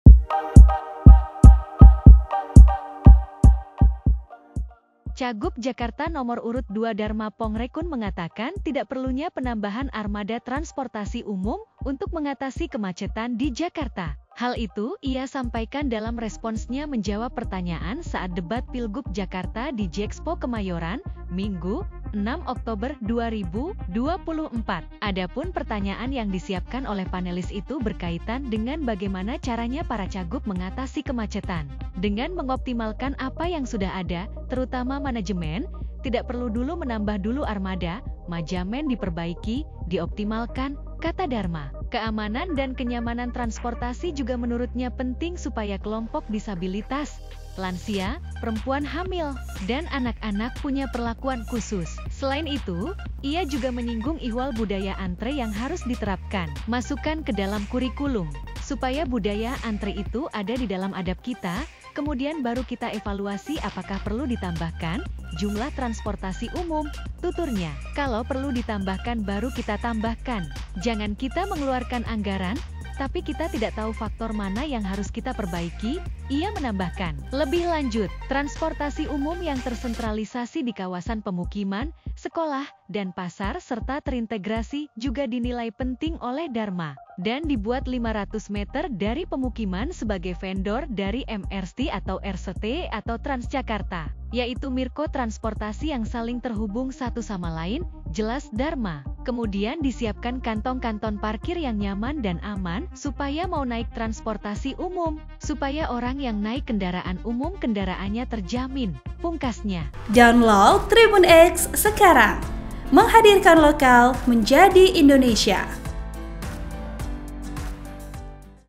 Solusi Kemacetan di Jakarta, Dharma Pongrekun Nilai Tidak Perlu Tambah Transportasi Umum
Cagub Jakarta nomor urut 2 Dharma Pongrekun mengatakan tidak perlunya penambahan armada transportasi umum untuk mengatasi kemacetan di Jakarta.

Hal itu ia sampaikan dalam responsnya menjawab pertanyaan saat debat Pilgub Jakarta di JIExpo Kemayoran, Minggu (6/10/2024).

Editor: Djohan Nur